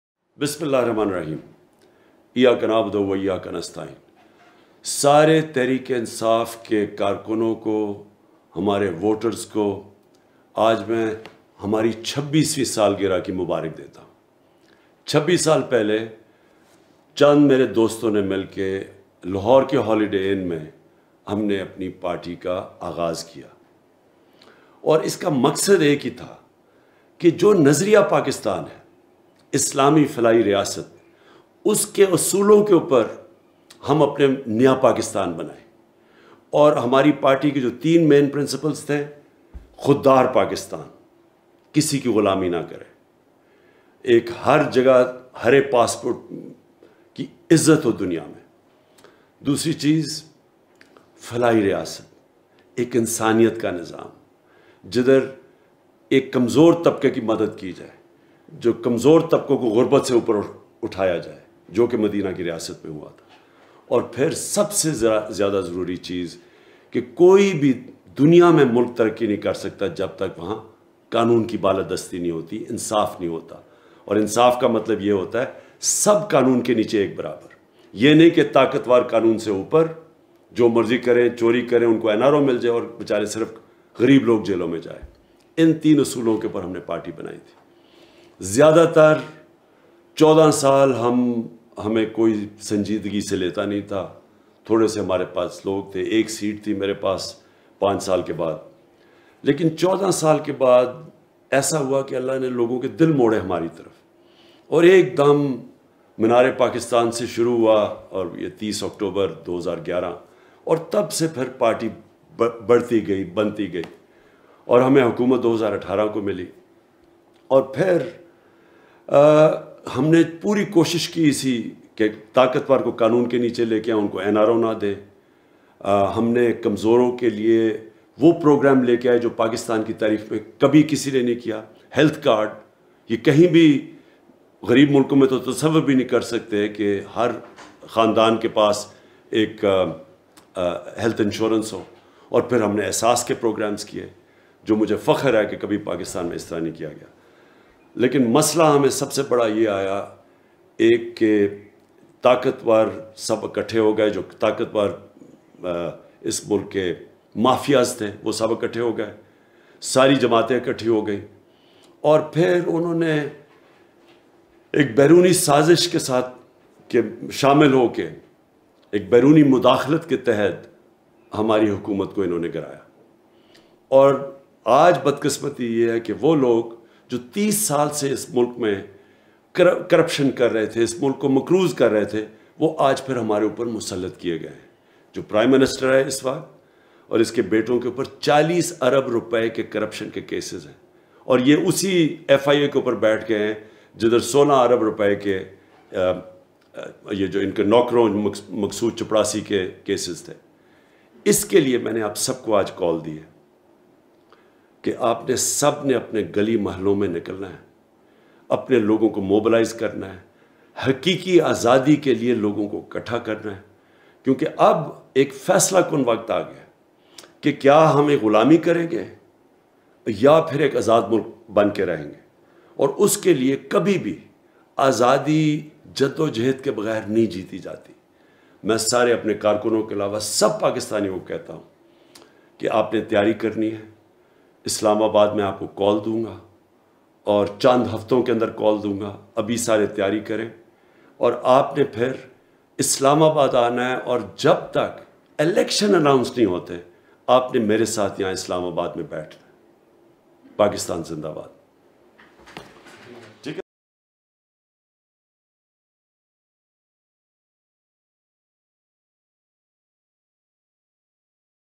Chairman PTI IMRAN KHAN Special Message For Nation On PTI Founding Day | News Today
News Today is a Leading News Channel in Pakistan. We Fully Updated You On Politics, Current Affair, Breaking News and Talk Shows.

NEWS TODAY.